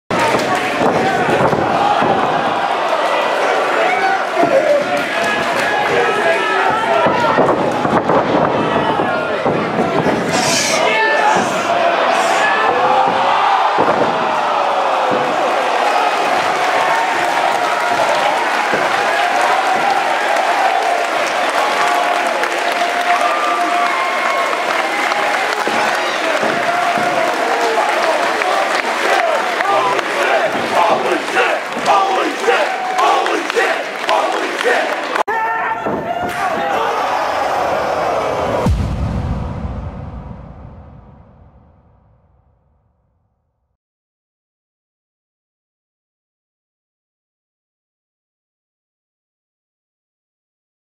Indy Wrestling: Mikey Webb shocks the Limitless fans after DVDing Ace Romero (Limitless Wrestling)
HOLY SH*T! HOLY SH"T! HOLY SH*T! Mikey Webb gives Ace Romero a devastating DVD and the fans of Limitless Wrestling lose their minds!

More clips from Limitless Wrestling coming soon. Get "Unreal" when it drops on dvd because this event from top to bottom was literally UNREAL!!